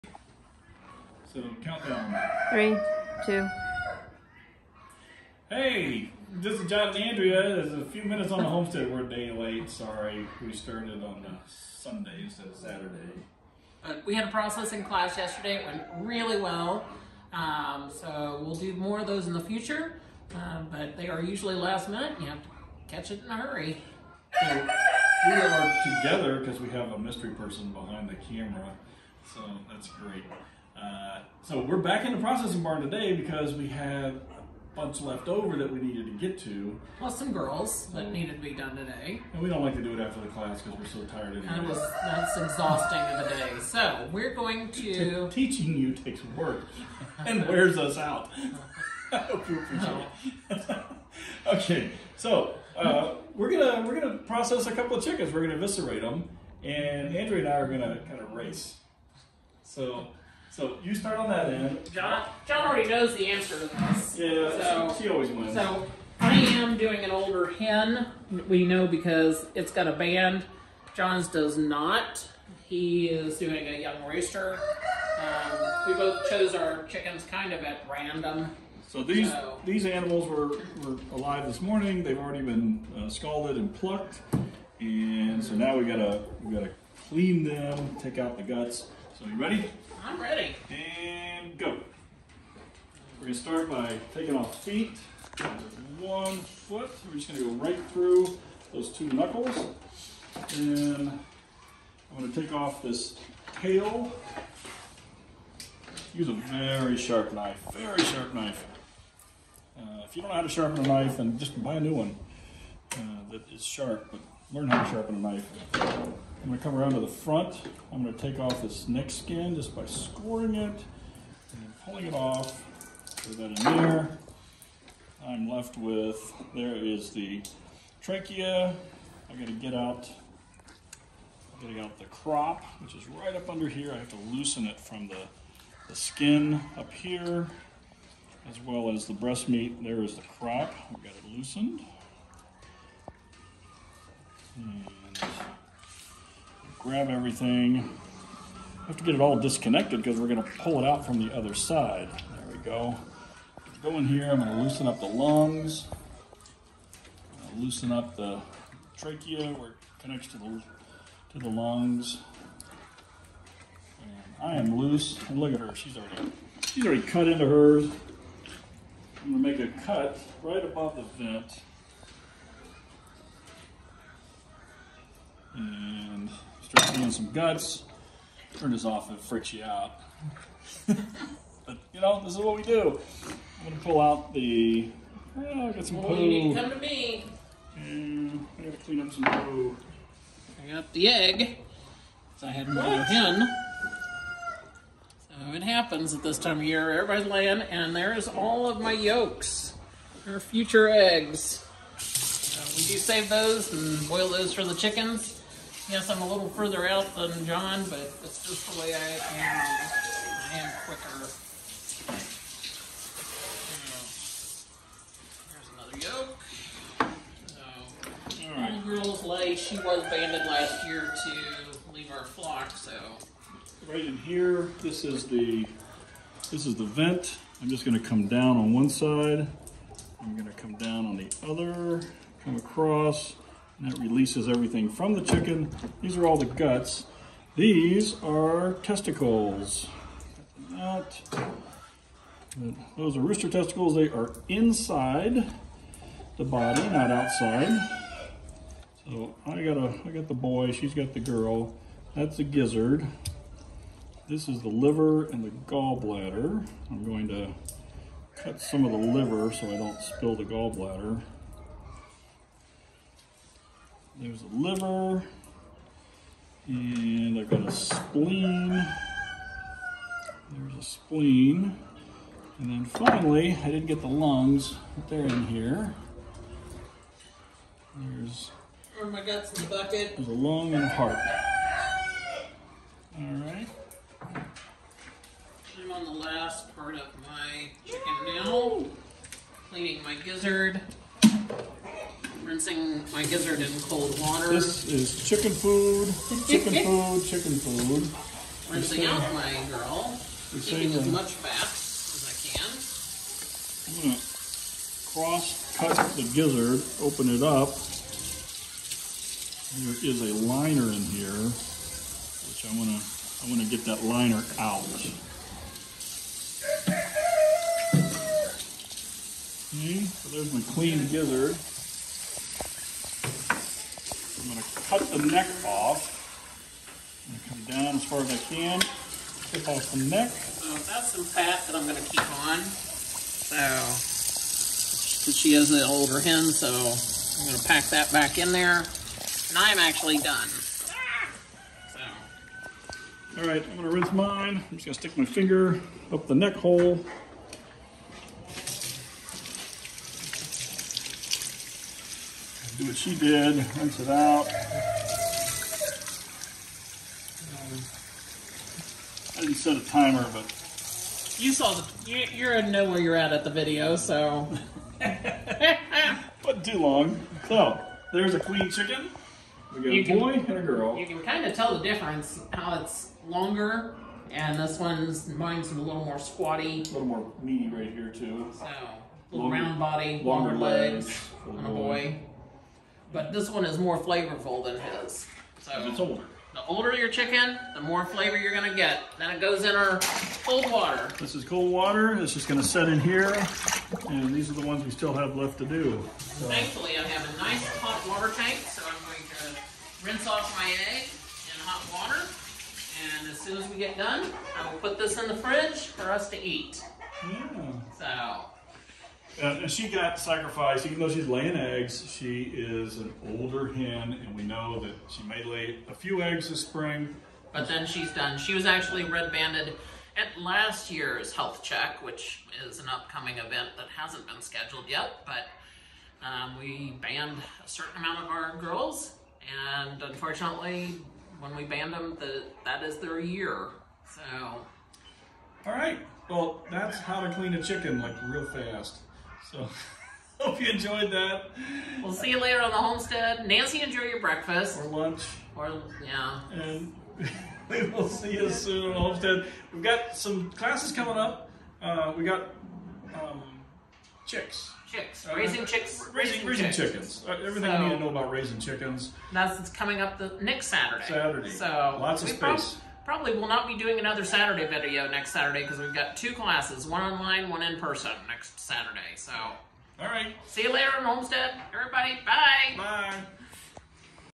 A Few Minutes on the Homestead - 26 February 2023
A quick look at what is happening on the farm. Today we’re processing chickens and Andrea and John race to see who is the fastest. Andrea always wins.